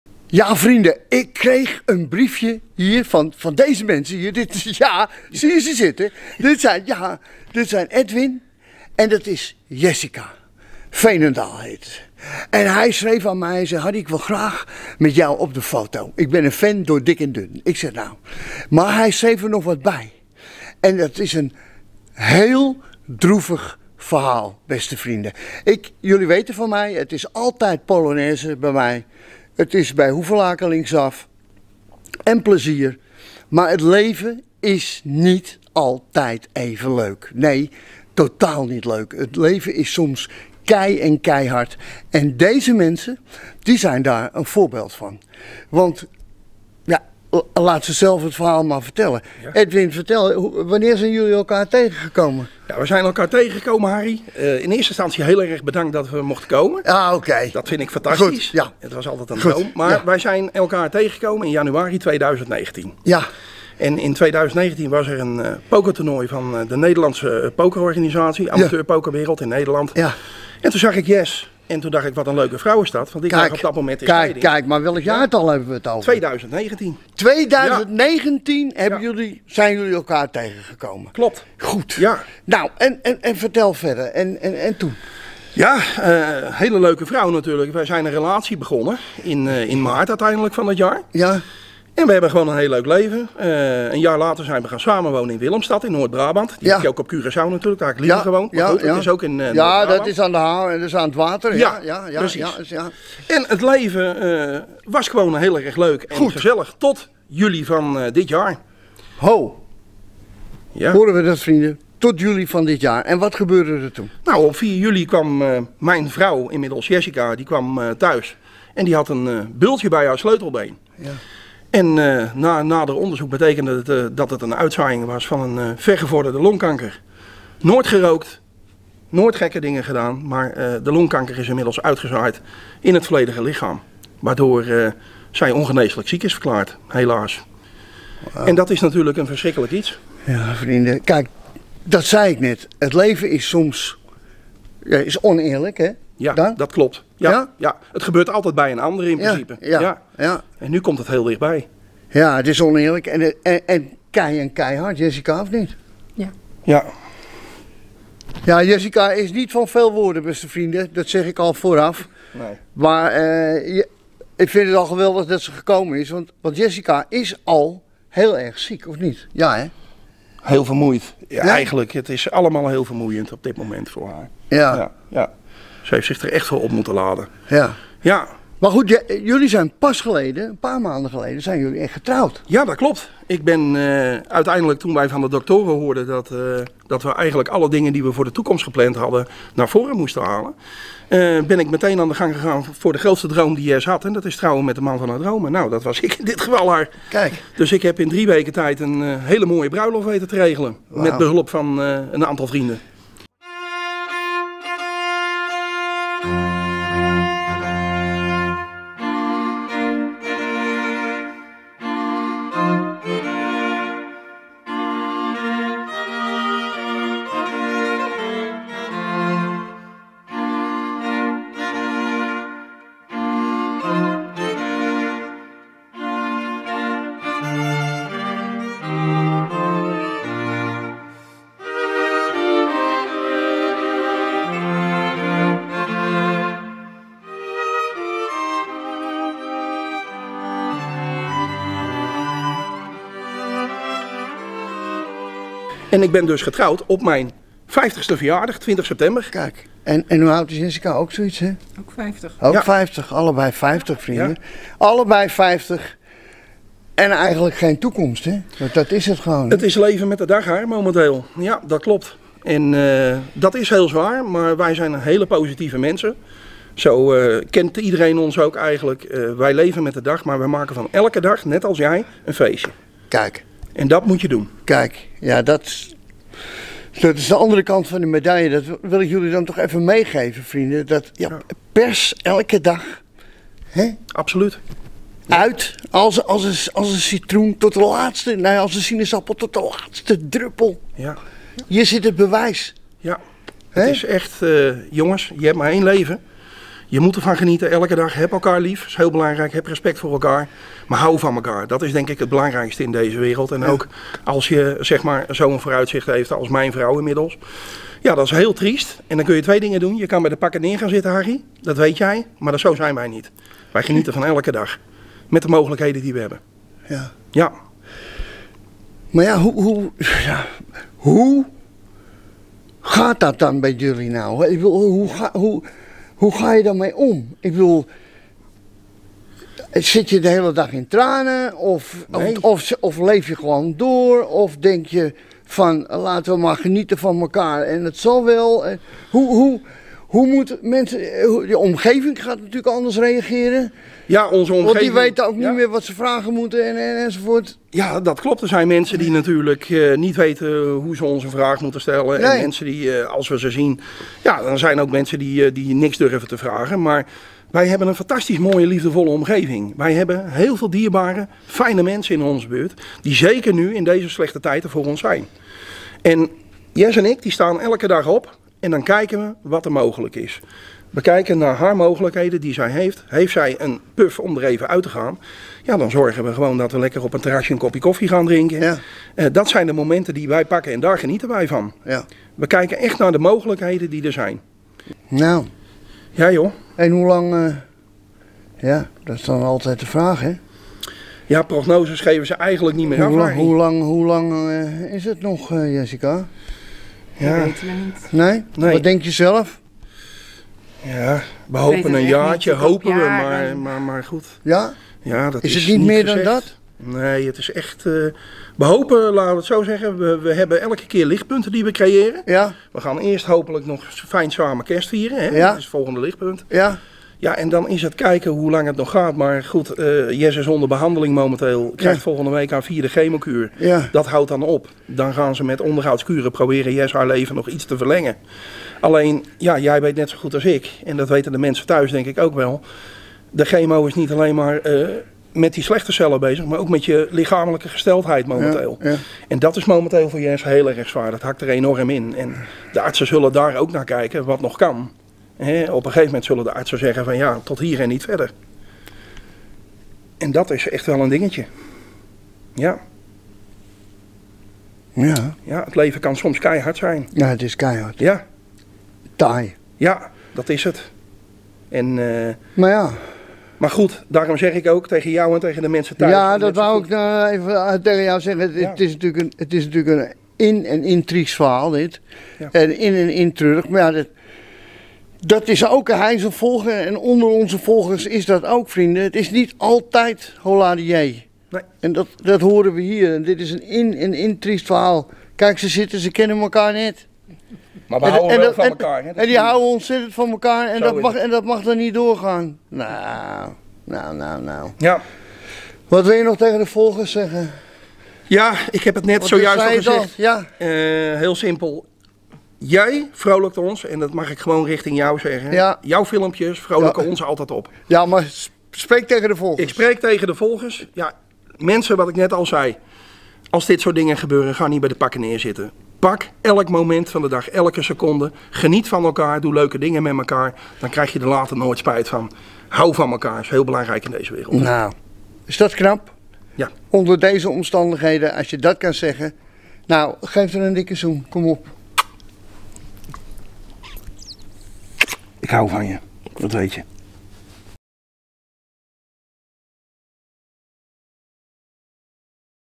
Een verdrietig verhaal.
Het leven kan keihard zijn. Dat vertellen Edwin en Jessica in deze video. Zij zitten in een hele verdrietige fase van hun leven, maar tegelijkertijd maken ze ons duidelijk dat we iedere dag van het leven moeten genieten!